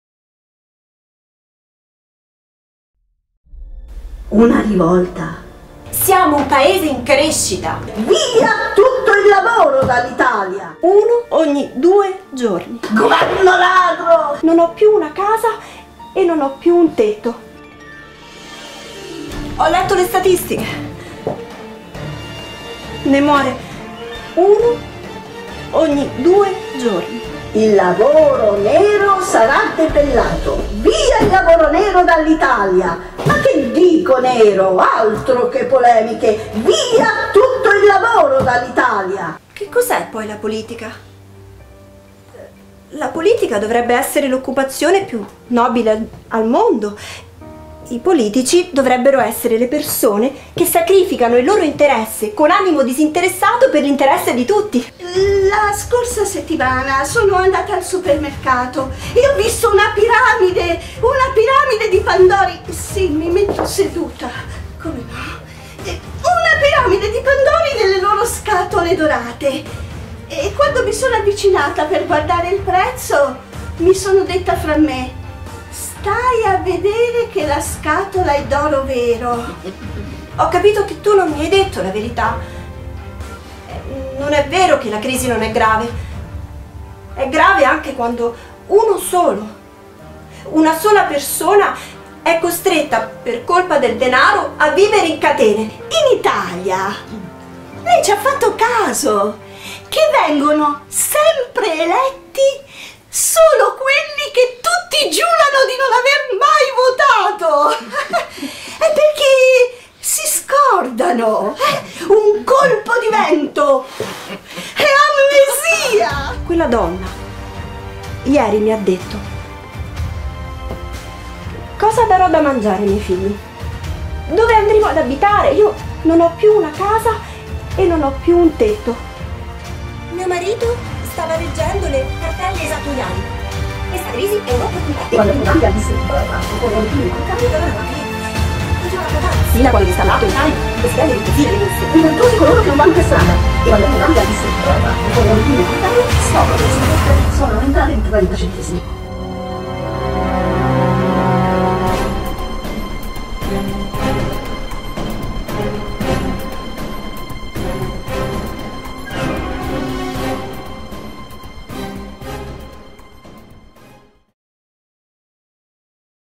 Fino al cielo risuona, con Alessia De Bortoli, Danilo Ianniello videoproduzione
Fino al cielo risuona, con Alessia De Bortoli ed Elisabetta Centore, scritto da Giselda Palombi. Regia di Elisabetta Centore.
Da un'idea di Alessia De Bortoli.